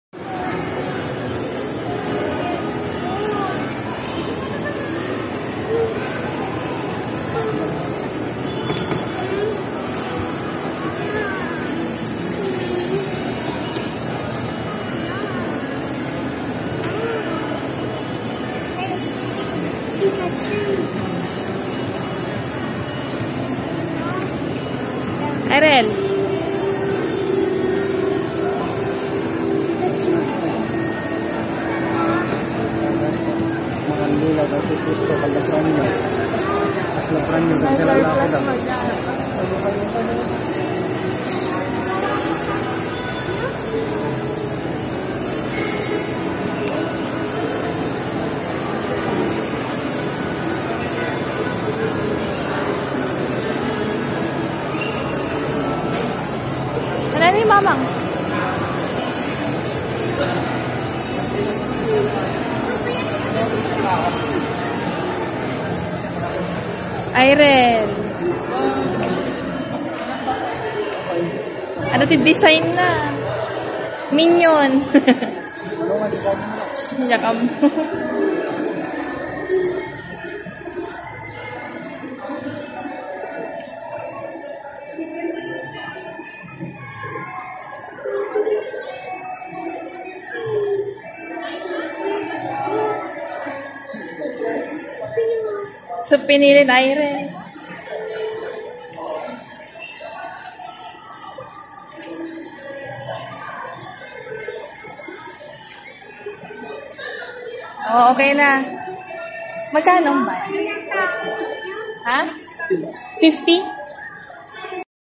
Cute Cotton Candy (Personalized Cotton Candy)
Yrel buying Personalized cotton candy
cartoon characters